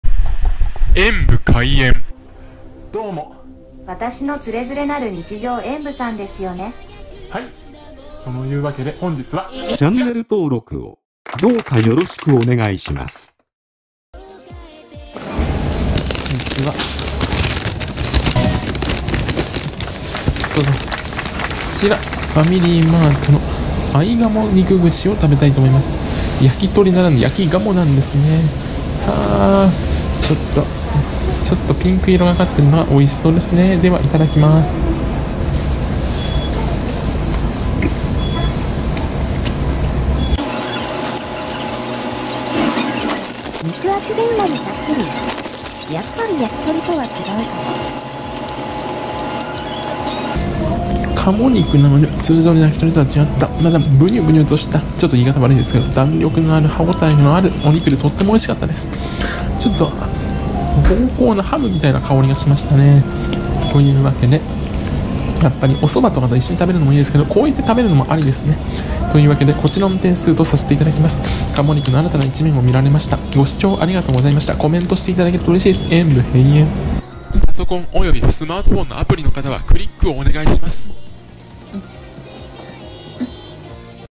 ファミリーマート 燻製鴨肉串が只の焼鳥とはまた違った美味しさがあった～
ファミリーマート　燻製鴨肉串を食べてみました。鴨肉の串って珍しい気がしますね～やっぱり只の焼鳥とは又違う！！
贈り物は此方まで↓
〒153-0042